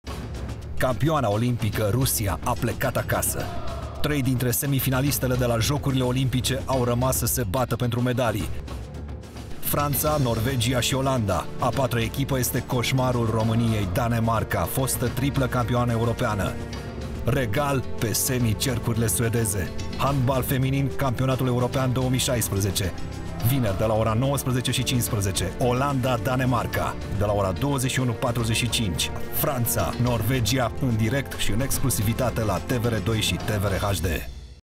Semifinalele CE de handbal feminin 2016, în direct la TVR2 şi TVR HD
Vineri, 16 decembrie, şi duminică, 18 decembrie, au loc ultimele meciurile de la actualul Campionat European de Handbal Feminin. Partidele sunt transmise în direct şi exclusivitate de Televiziunea Română, la TVR 2 şi TVR HD, astfel:

În 16 decembrie:
ora 16:45, locul 5/6:  România – Germania;
ora 19:15 Semifinala 1: Olanda – Danemarc;a
0ra 21:45 Semifinala 2: Franţa - Norvegia.

În 18 decembrie, de la 16.30 Finala mică şi de la 19.00 Finala.